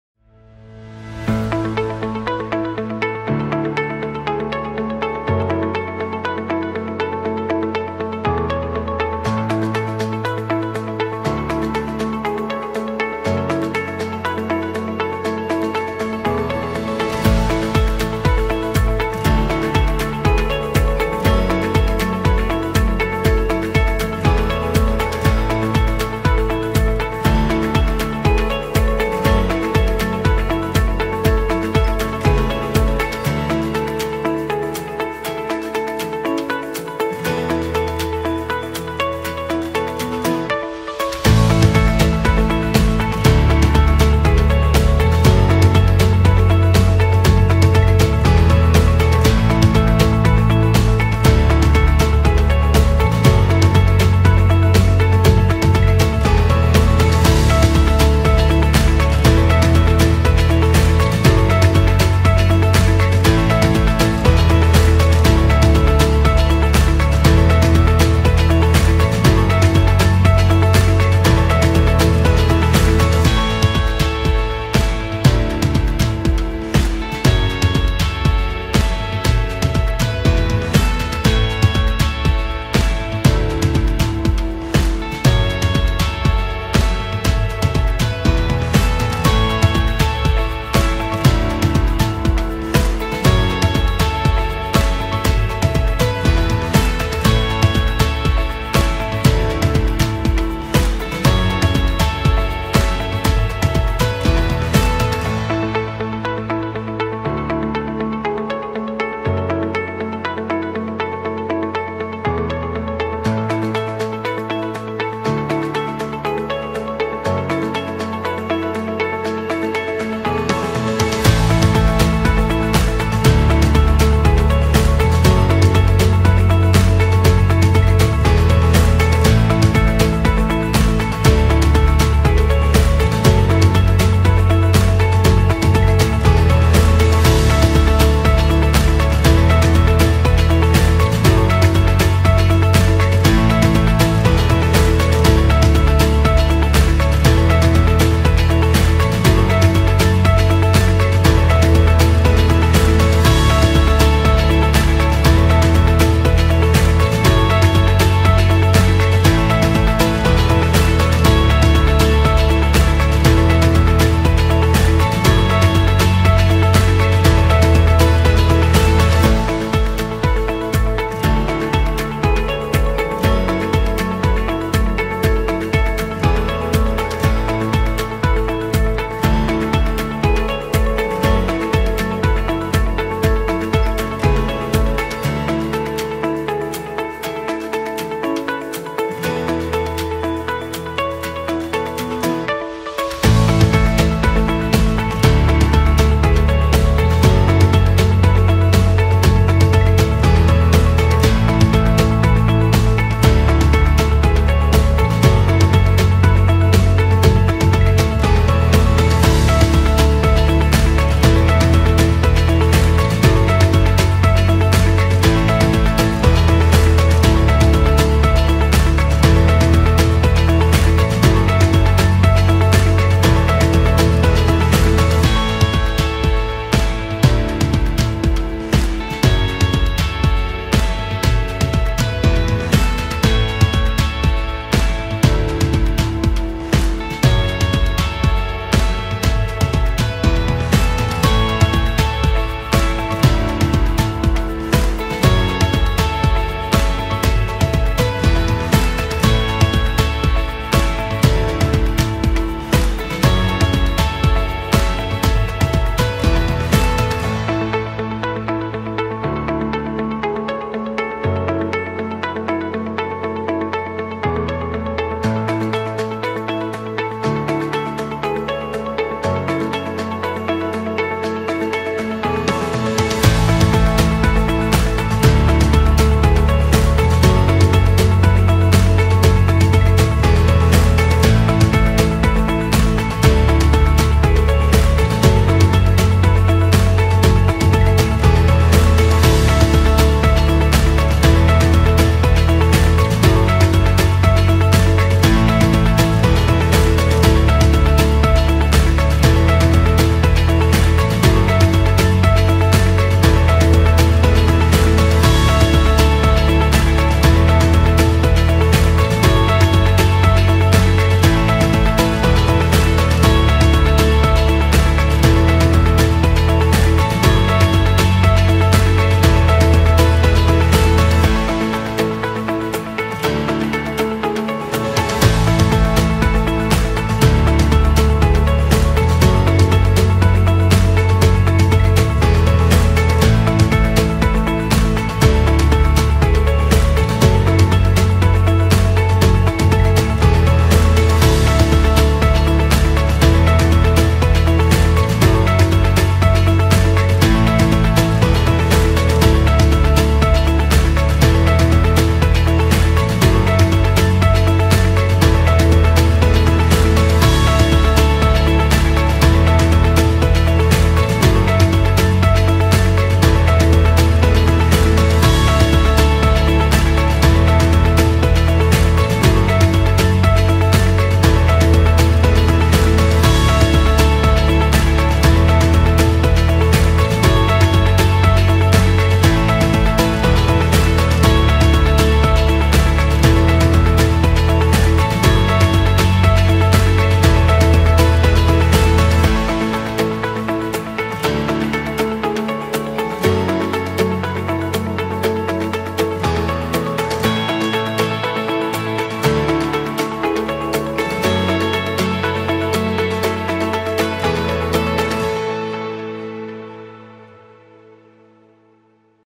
Vinnitsa National Medical University — my university
Vinnitsya national memorial pirogov university is one of the best colleges in Ukraine so far with great infrastructures and so on...